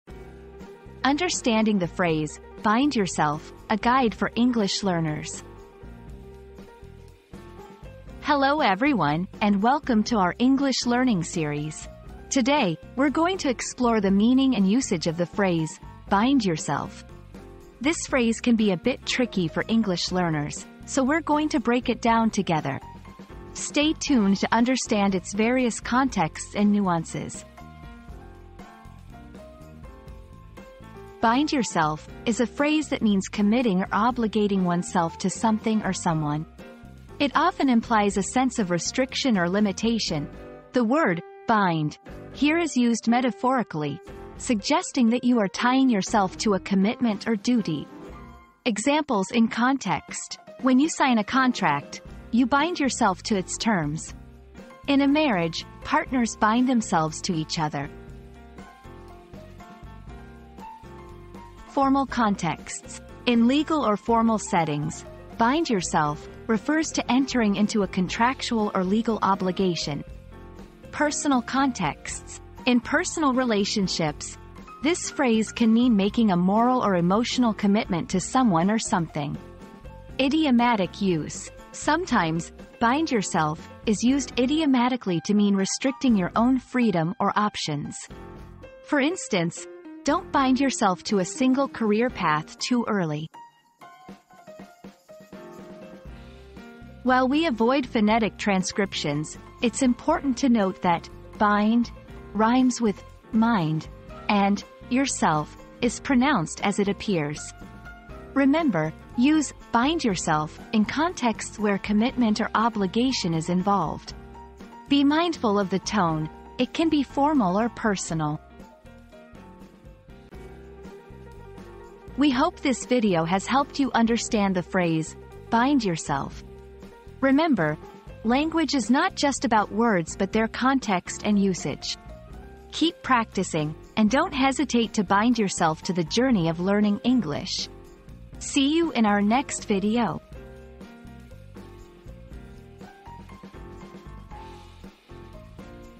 Understanding the Phrase "Bind Yourself": A Guide for English Learners
00:00 • Introduction - Understanding the Phrase "Bind Yourself": A Guide for English Learners
00:30 • What Does "Bind Yourself" Mean?
01:01 • Different Contexts and Uses
01:36 • Pronunciation and Usage Tips